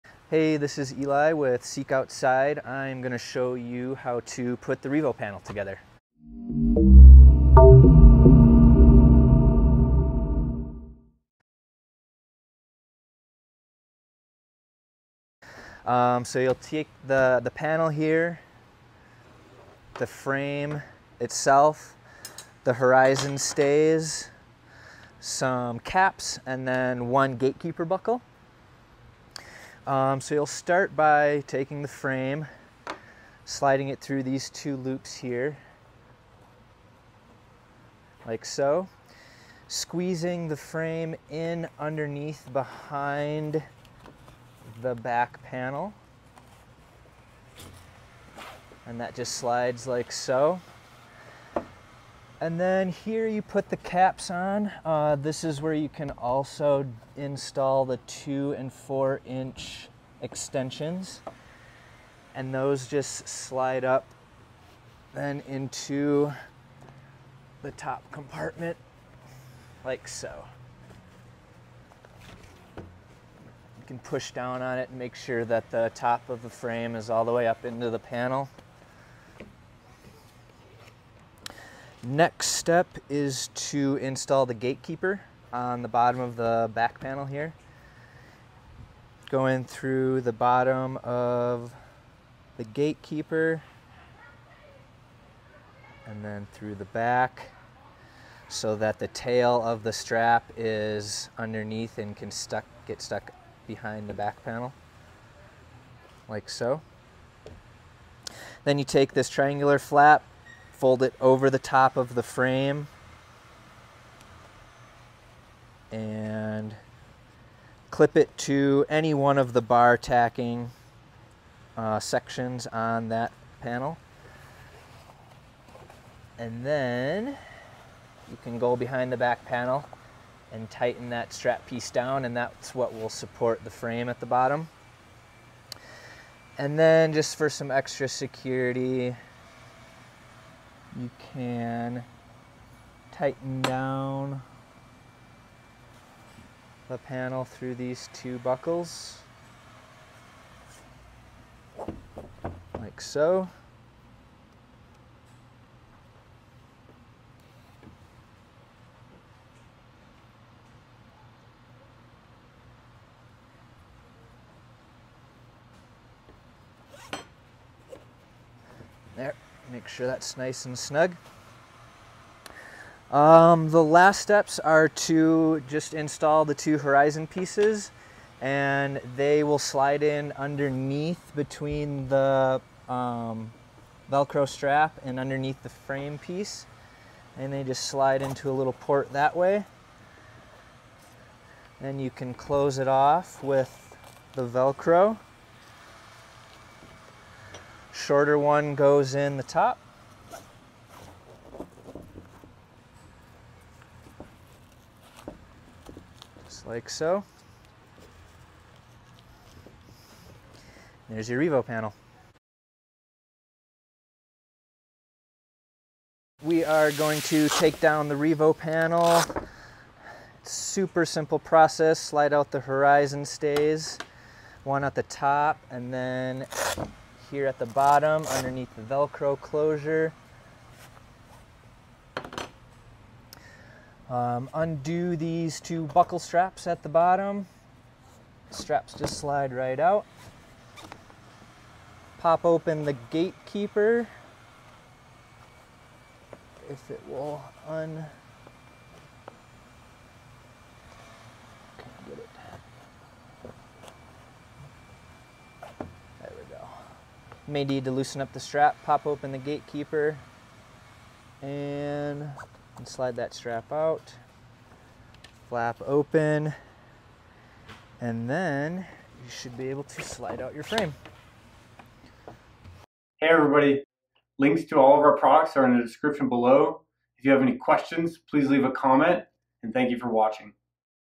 Seek Outside Revolution Pack Frame Panel Assembly And Disassembly
In this video, Ely walks us through how to assemble and disassemble a Revolution Pack Frame Panel. This is helpful if you want to switch out a frame from one of our Integrated packs into a Breakaway pack that uses the Revolution Panel or vice versa. Also, this can be helpful if you want to wash your panel or simply want to add 2" or 4" extensions.